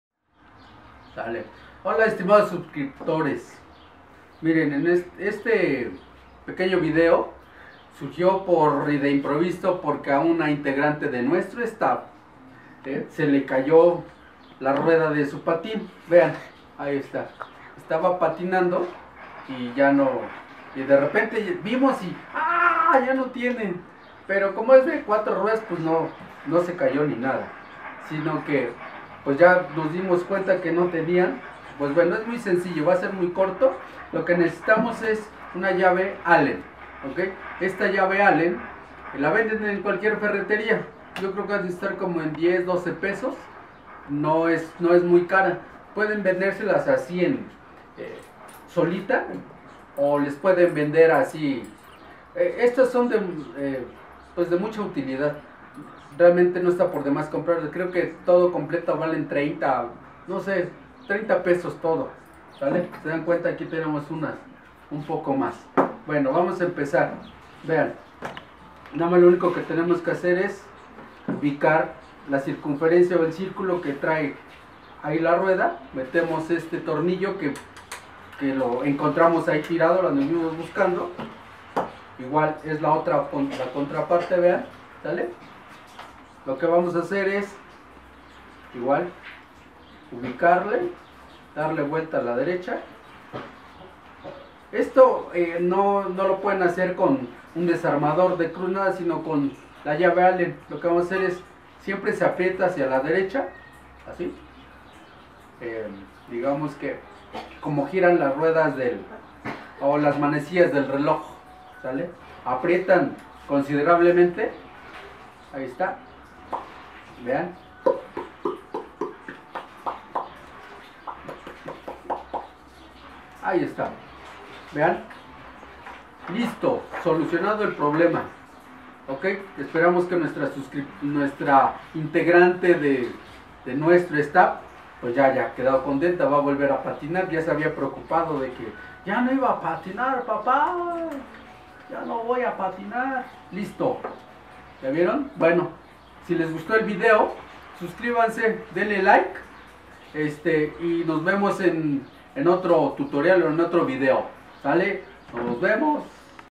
COMO ARREGLAR CON $20 TUS PATINES LINEALES
HOLA AMIGOS ESTE VIDEO ES PARA QUE SIGAS PATINANDO A GUSTO CUANDO SE TE CAIGA LA RUEDA DE TUS PATINES Y CON $20 PUEDAS SOLUCIONARLO: CHELYS